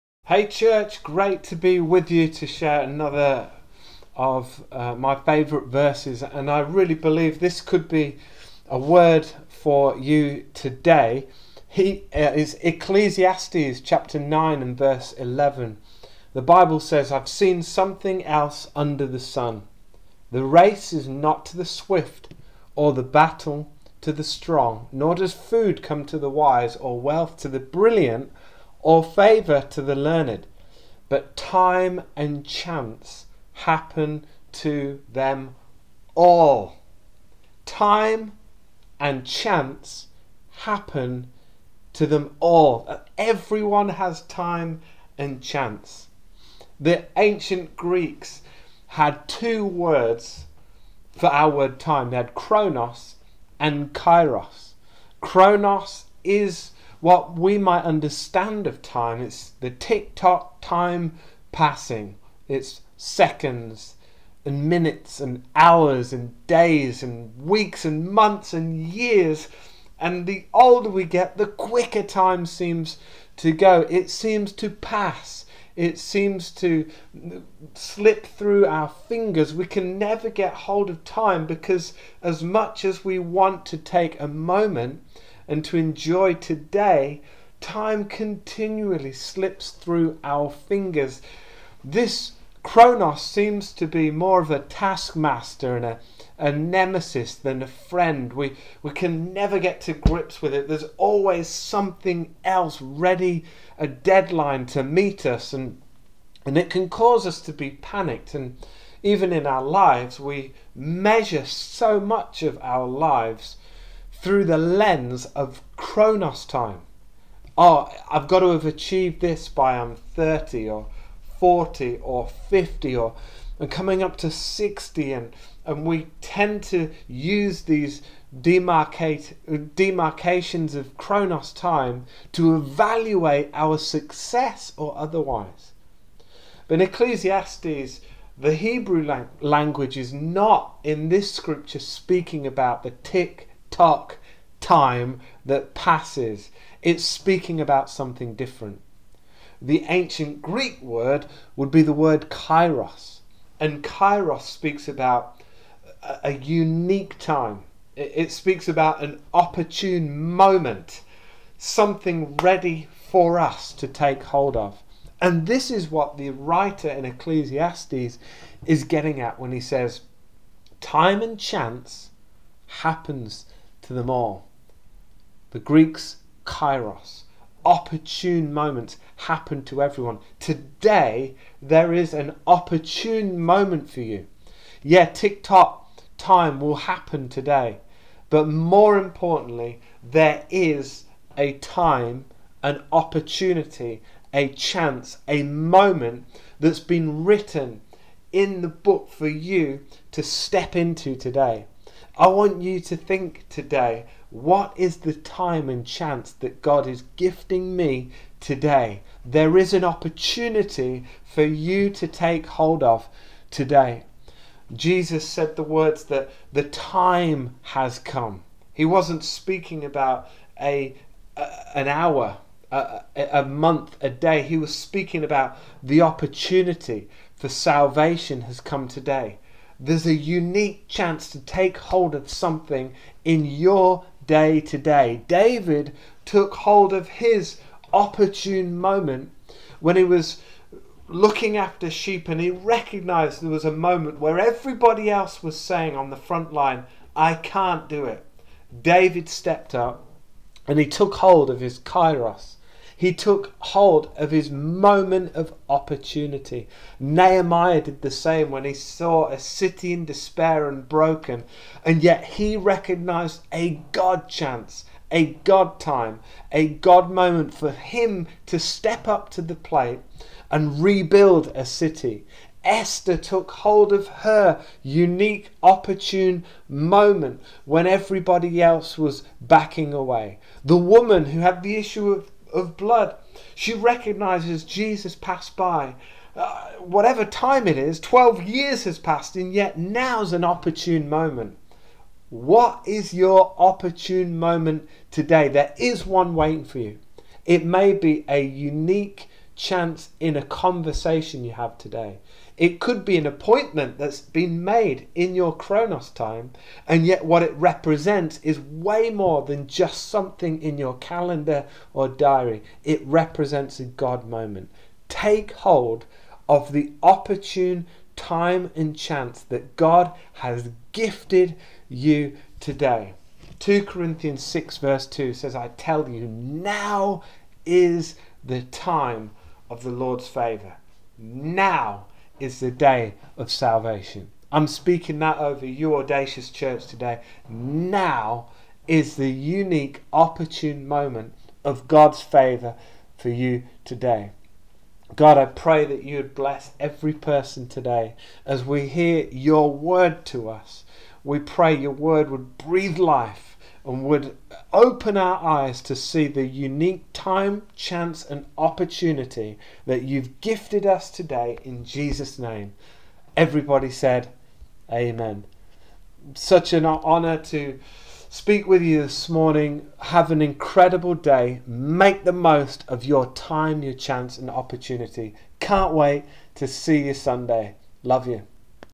!Audacious Daily Devotionals - Monday 20th September 2021
Today, Pastor Mark Foster shares a Bible passage that has great meaning to him.